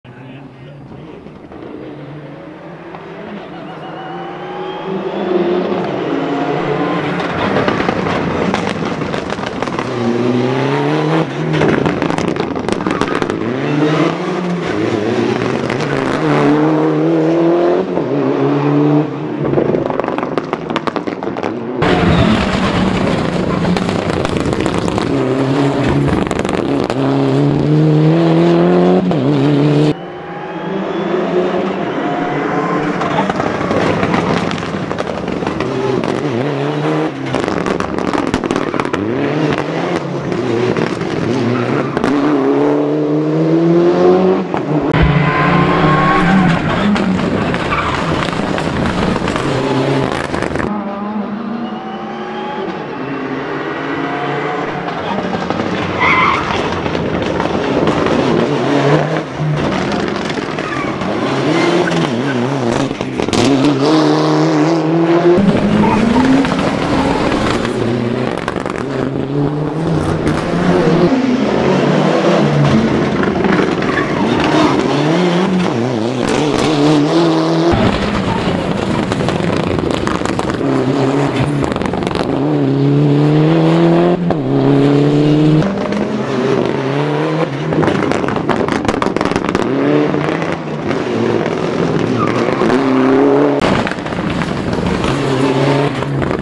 Fahrzeug #1 mit M. Bieg / C. Nemenich im Mitsubishi Lancer Evo IX (uncut)
In diesem Video seht ihr das Team M. Bieg / C. Nemenich im Mitsubishi Lancer Evo IX. Das Video ist unbearbeitet und kann kostenlos genutzt und weiterbearbeitet werden.

Es war mal wieder ordentlich was los auf dem Fischereihafen in Bremerhaven. Denn am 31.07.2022 fand dort die

49. Bremerhavener Rallye-Sprint 2022 und
50. Fischereihafen Rallye-Sprint 2022

und parapelle die

19. Bremerhavener Retro-Rallye-Sprint 2022 und
20. Fischereihafen Retro-Rallye-Sprint 2022